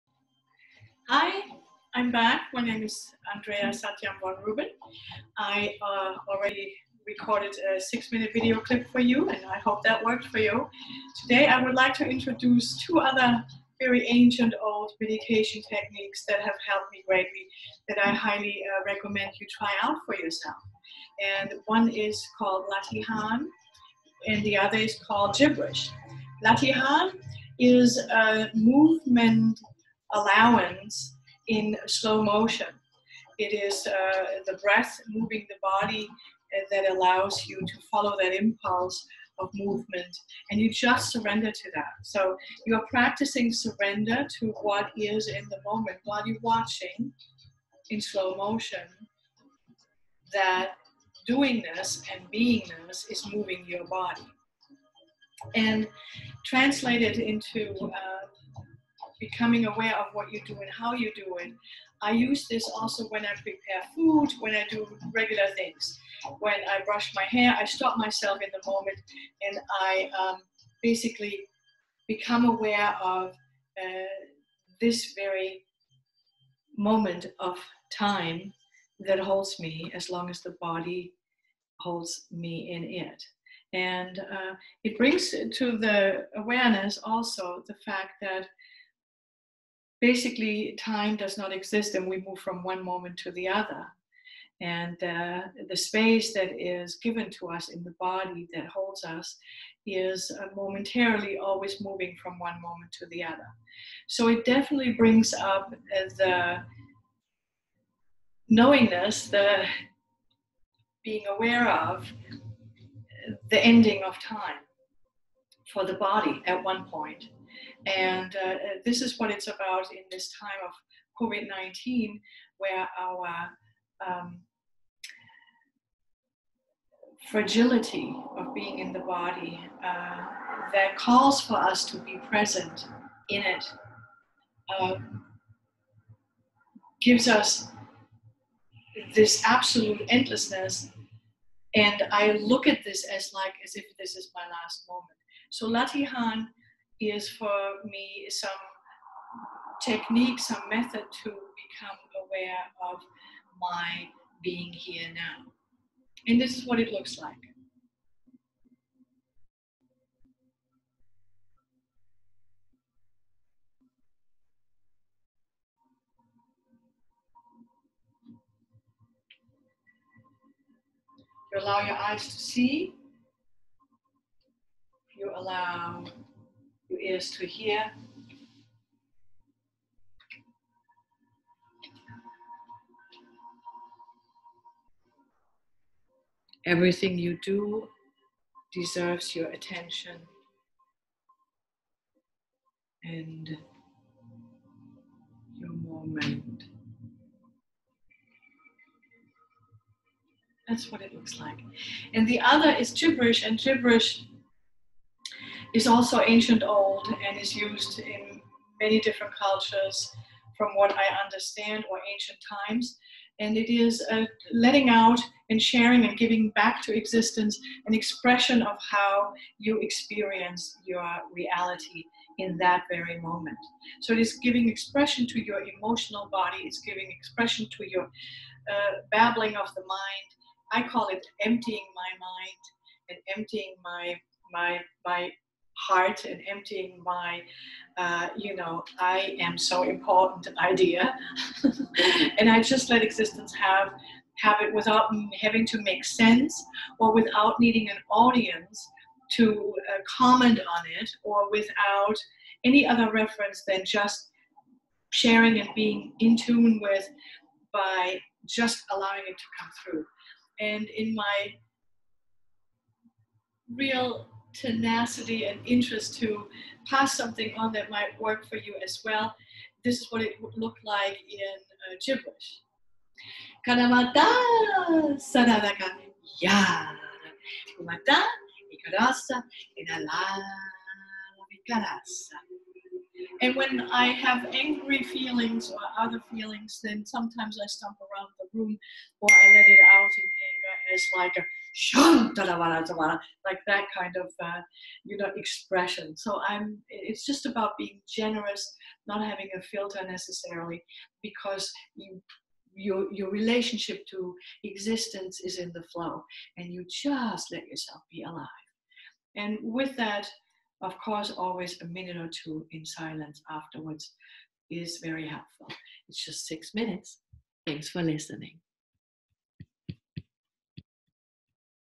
Meditation - Latihan & gibberish for practicing presence (6min)
Latihan & gibberish for practicing presence a short meditation
Conscious Transformation with Andrea Satyam Von Rueben.